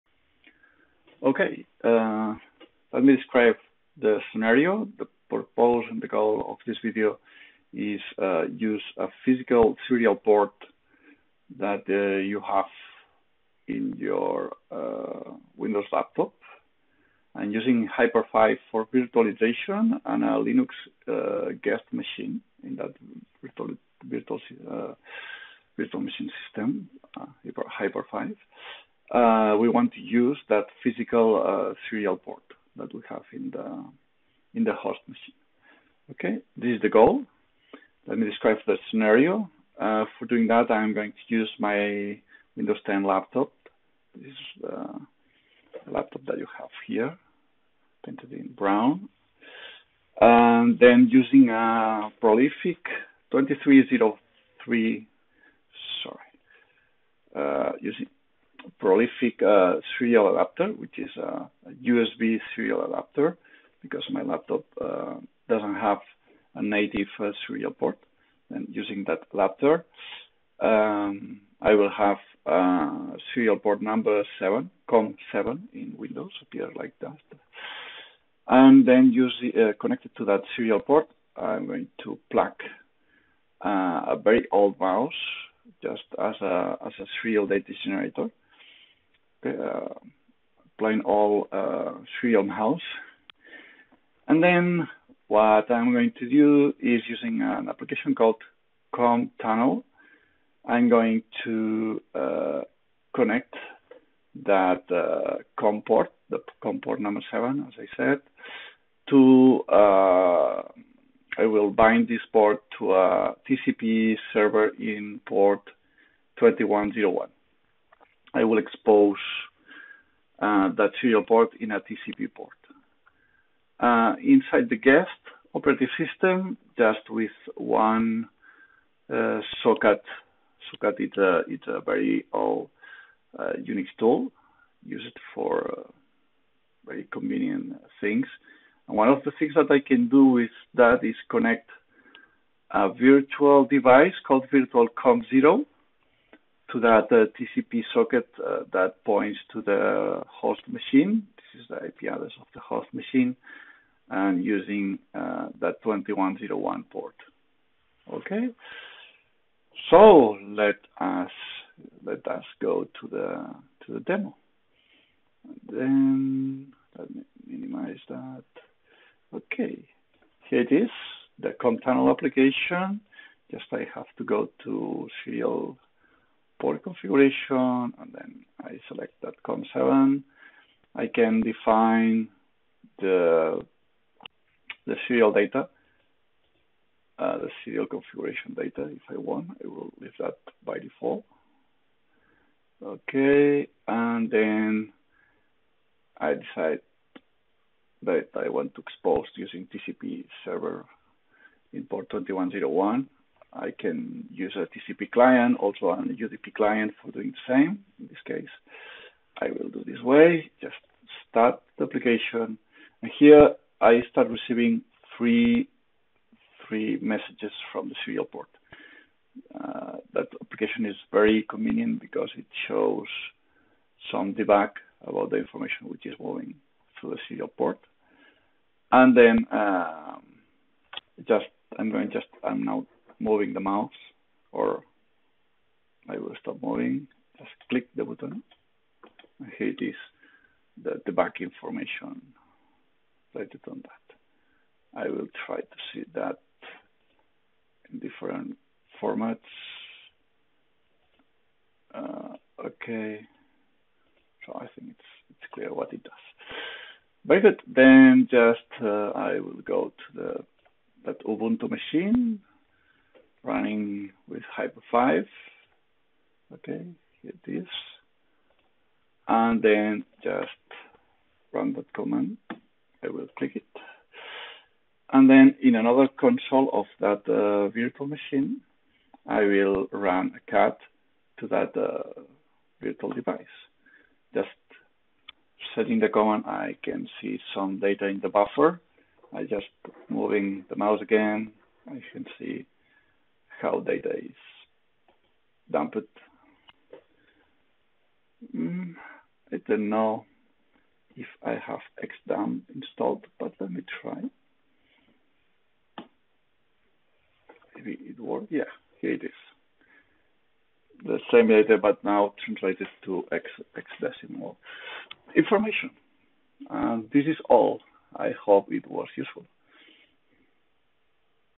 Hyper-V serial port howto
How to connect a serial port on the host machine of Microsoft Hyper-V to a Linux guest machine using a TCP socket.

Applications used:
* socat for linux